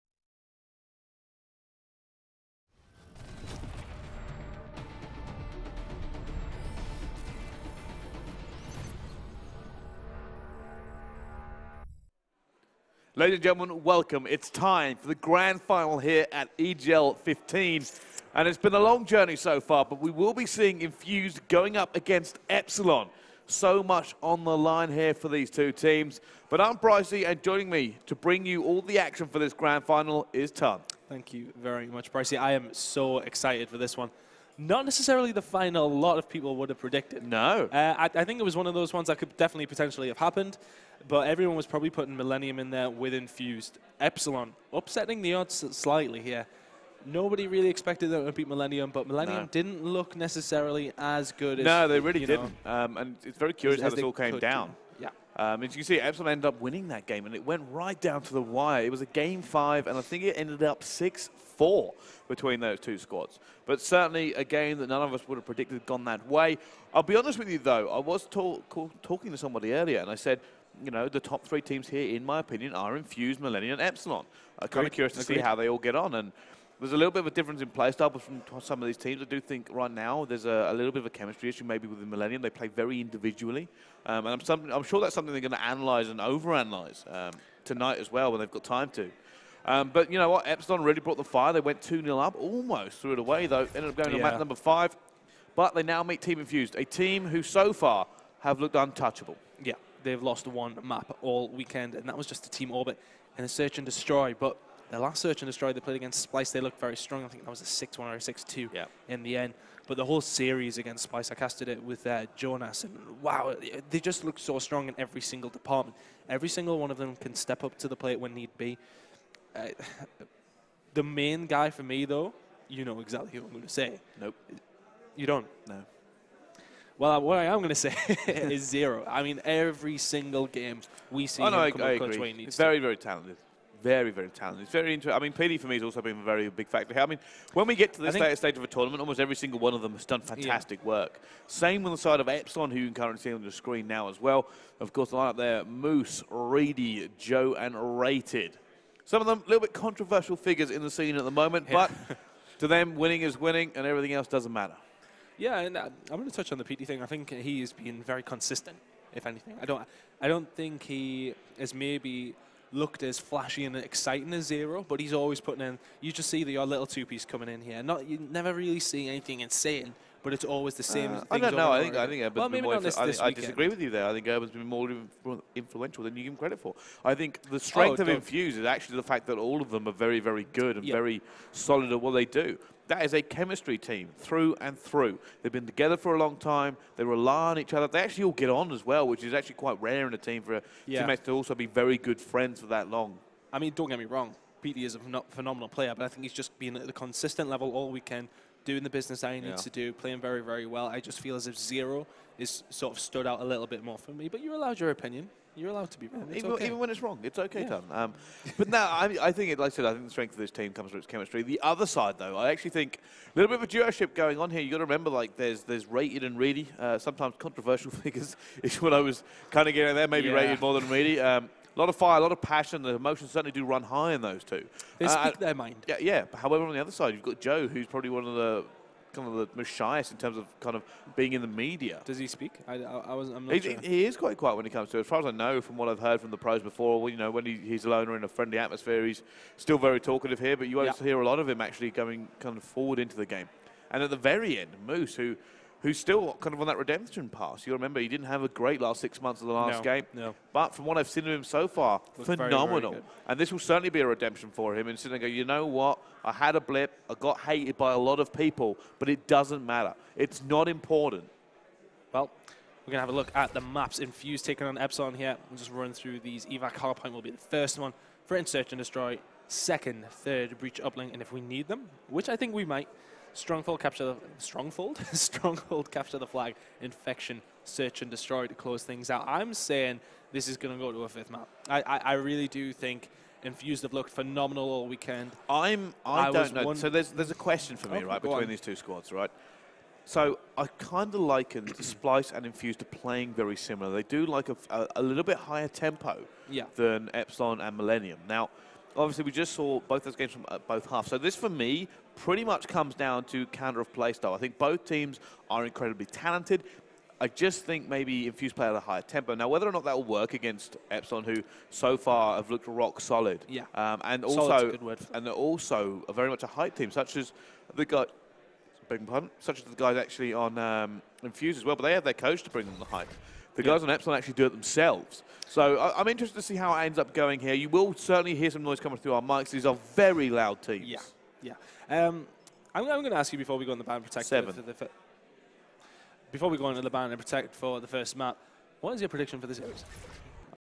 EGL Open - London 2016: Epsilon vs Team Infused: Final - Intro
Footage recorded live at European Gaming League’s 15th event, EGL Open - London 2016 at the Old Truman Brewery, London, UK.

Supported by:
Scuf Gaming
ASTRO Gaming
IIyama
ManCave
Lime